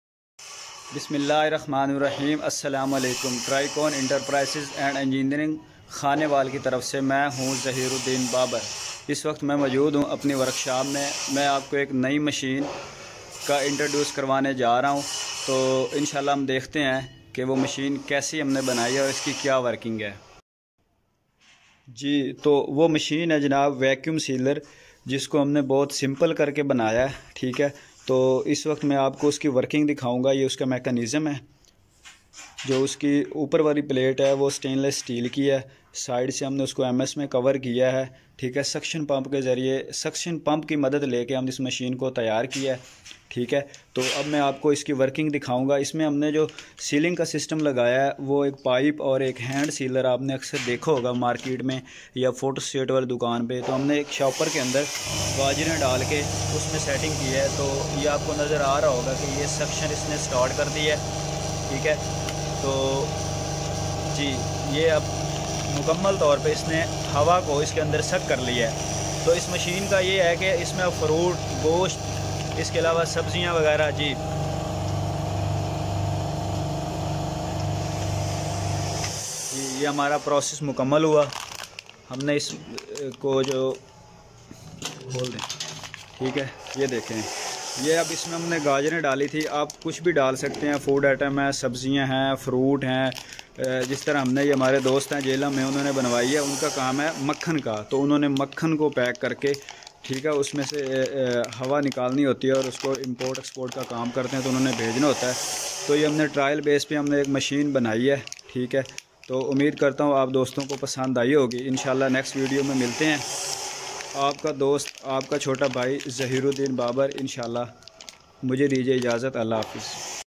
Vecum sealer for air suction 03005148023
Vecum sealer very simple. For air suction
Vegetables. Meat dry fruitd etc.
For more details
Zaheer ud Din babar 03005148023